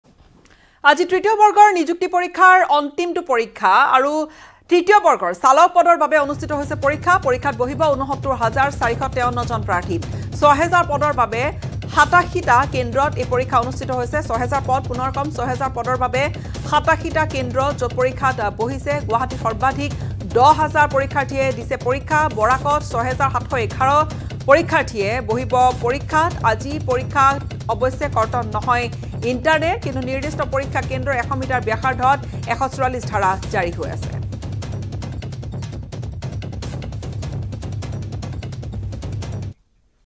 Assam Direct Recruitment | আজি ৩য় বৰ্গৰ অন্তিমটো পৰীক্ষা | Assam News
Assam Direct Recruitment 2022 : Political support for whistleblower Victor Das pours in after he becomes the face of one of the most encompassing scams that could have ever taken place under the new government under CM Himanta Biswa Sarma’s leadership.

Latest News | News18 Assam Northeast | Assam News | News in Assam | Assamese News | Northeast news | Northeast India |